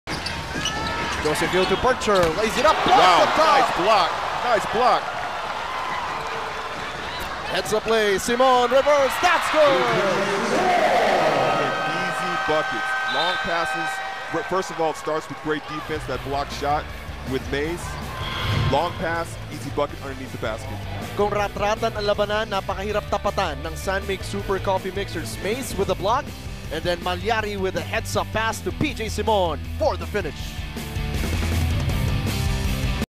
James Mays Block lead to PJ Simon Layup|PBA
Air21 Express vs San Mig Coffee April 9 2014 PBA Comissioners Cup